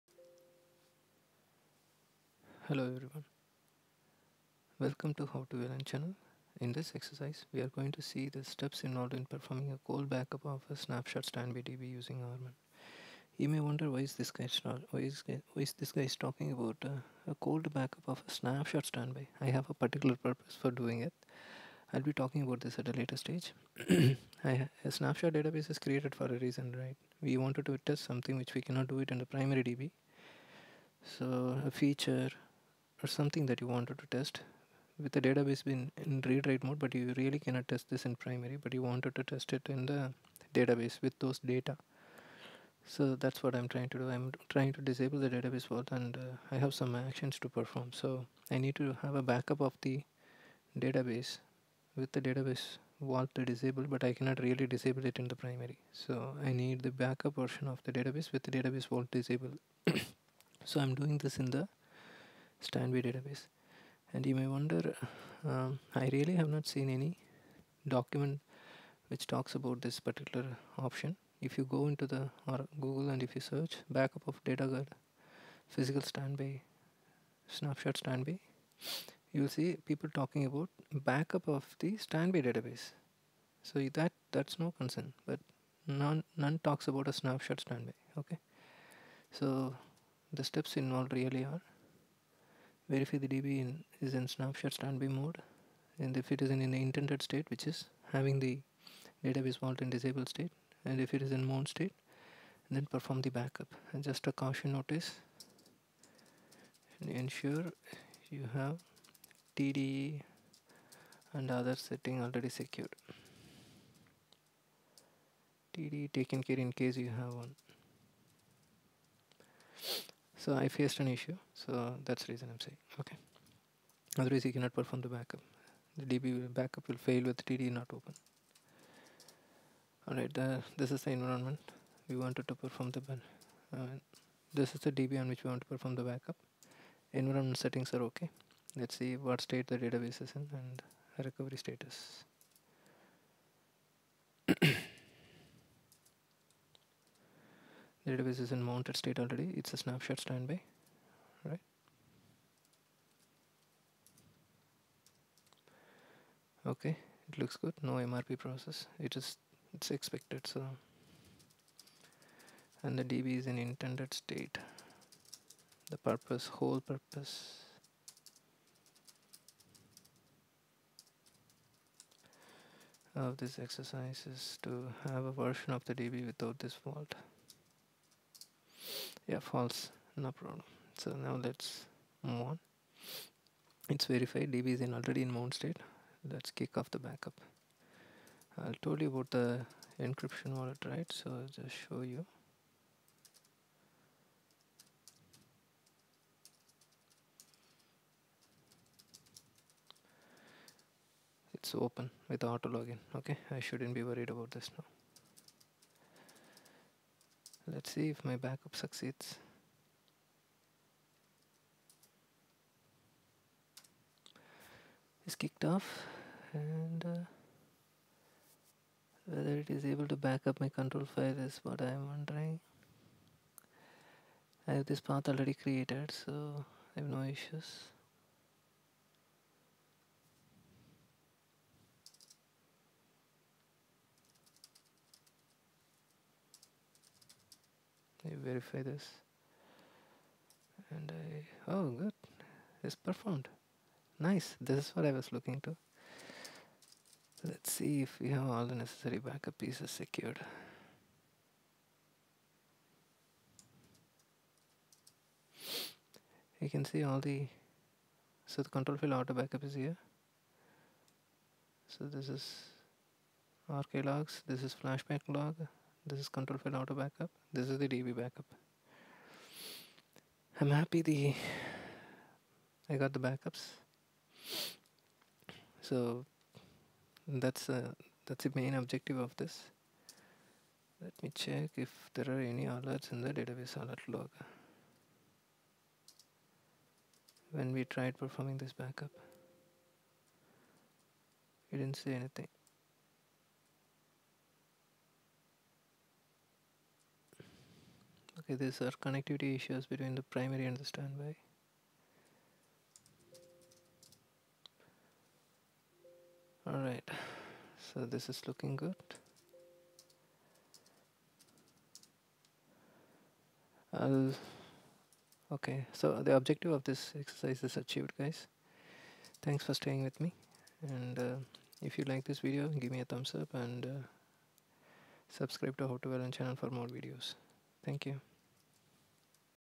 Steps to perform a cold backup in oracle snapshot standby database
Dear Viewers,

In this video, we will see the steps involved in performing a cold backup in oracle snapshot standby database.

Thanks